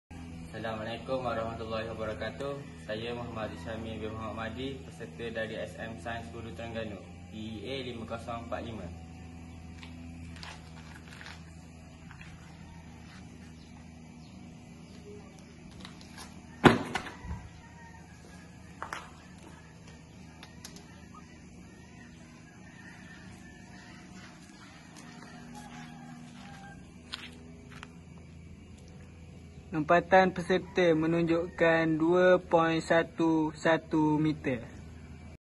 Episod 133 : PERTANDINGAN KEMAHIRAN SUKAN OLAHRAGA DALAM TALIAN PERINGKAT NEGERI TERENGGANU
MUHAMMAD HAZIQ SYAHMI BIN MUHD MARDHI DARI SM SAINS HULU TERENGGANU TELAH  MEWAKILI DAERAH HULU TERENGGANU DALAM PERTANDINGAN KEMAHIRAN SUKAN OLAHRAGA DALAM TALIAN PERINGKAT NEGERI TERENGGANU